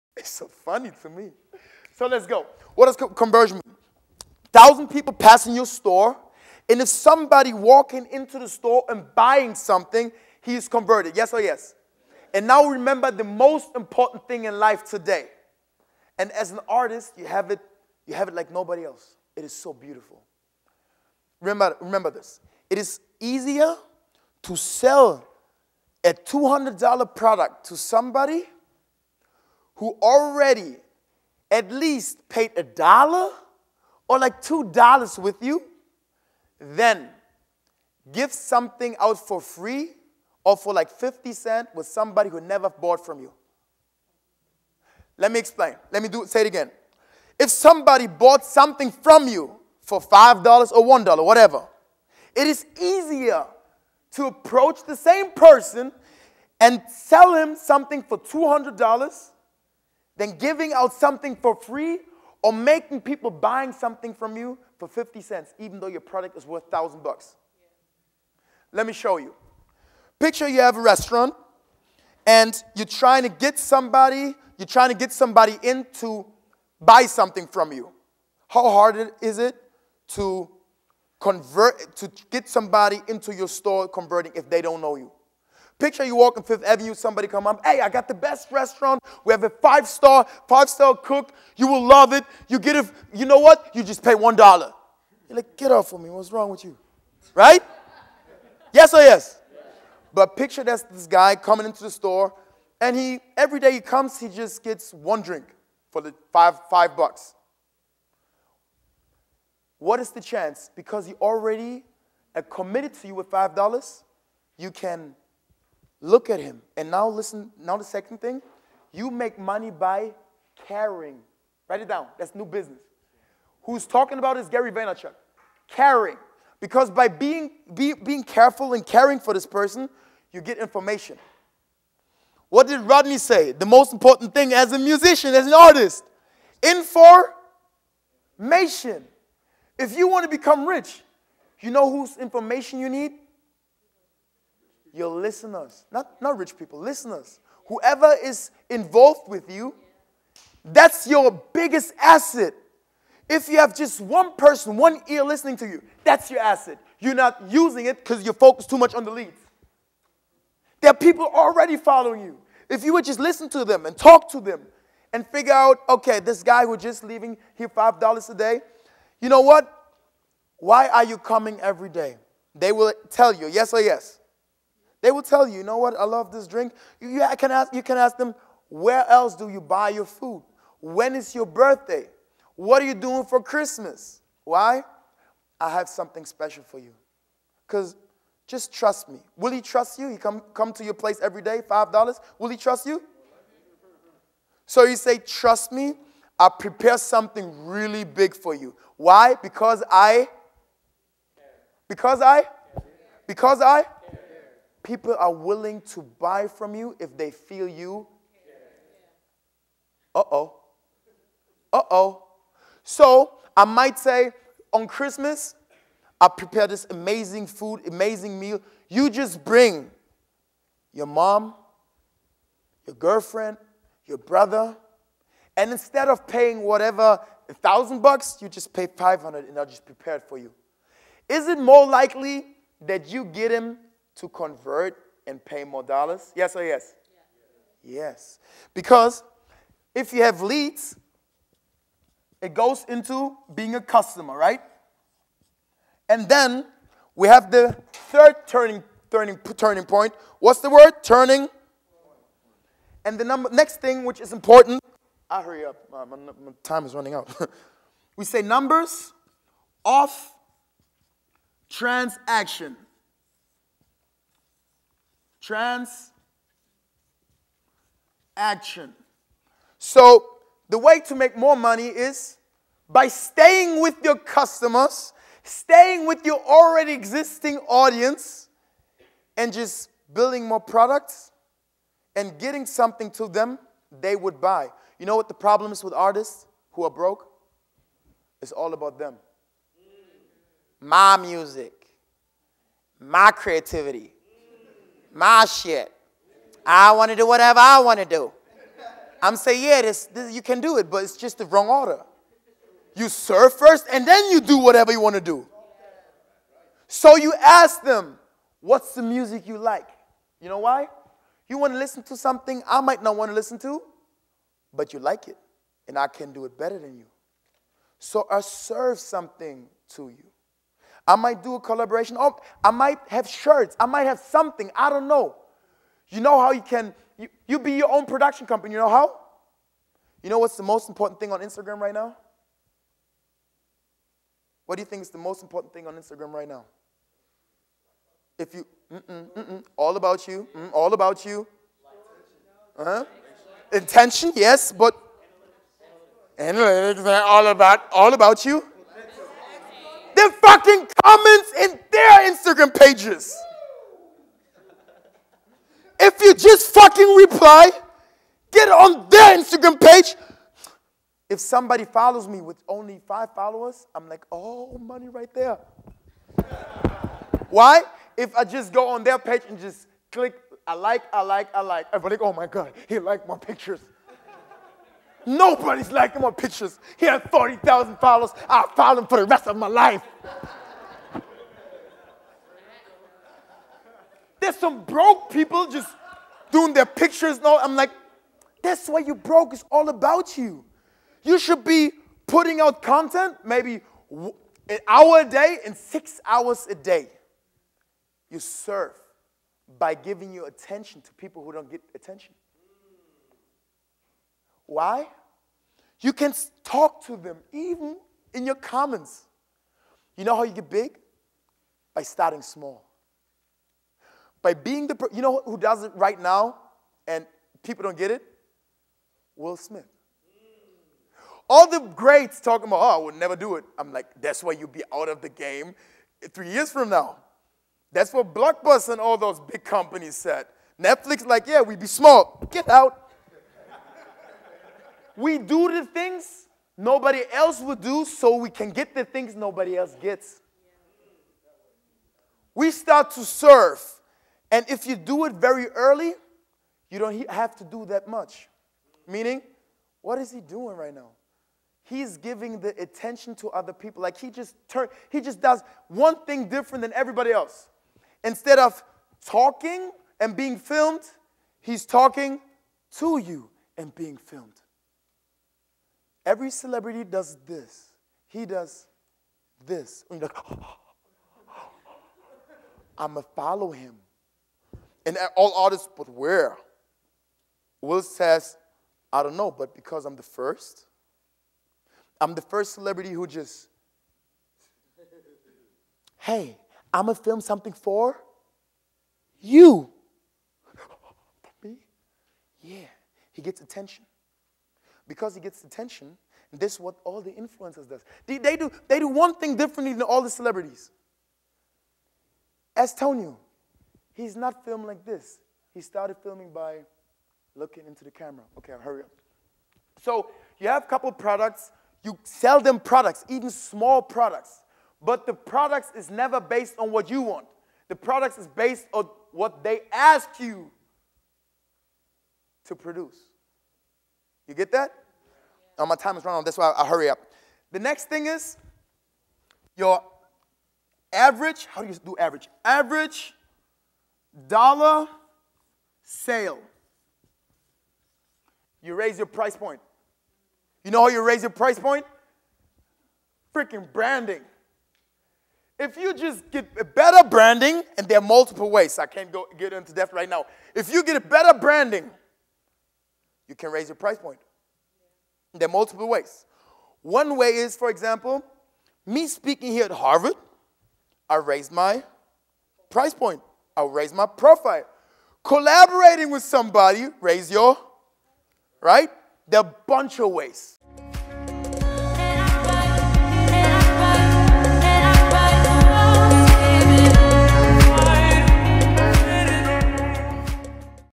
Thaddeus Koroma - Maintaining Your Audience !
Music Entrepreneur Conference 2018
Harvard University Science Center

Music Entrepreneurs want a chance to fly, but don't know how to get their wheels off the ground. In 201,  we took over Harvard University Science Center bringing legends, leaders and innovators. We revolutionized the artists' role in the business of music. In 2019, we will change the culture forever.